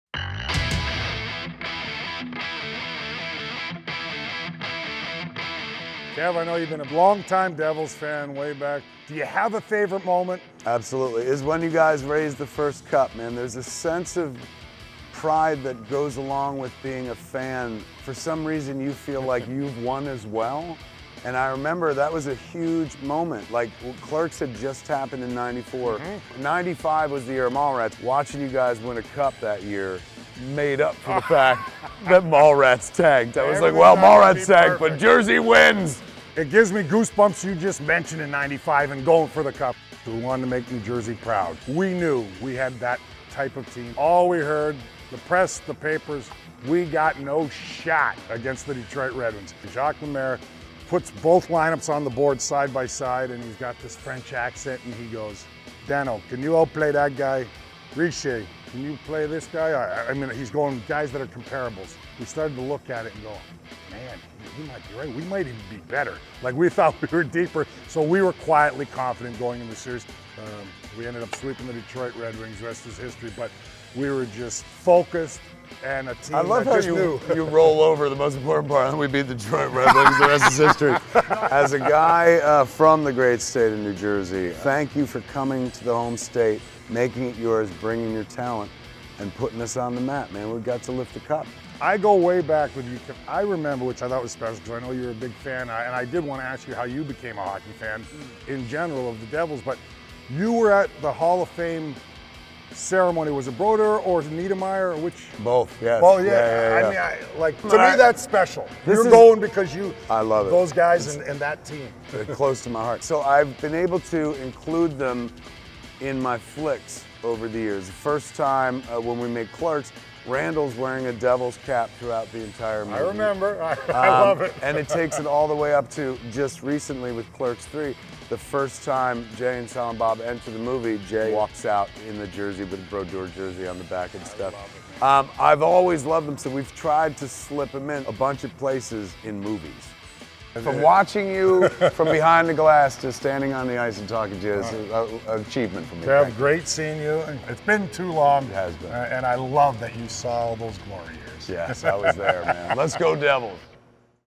Ken Daneyko and Kevin Smith Chat Devils
Ken Daneyko and Kevin Smith chat about the Devils and Dano's career/40 years with the franchise.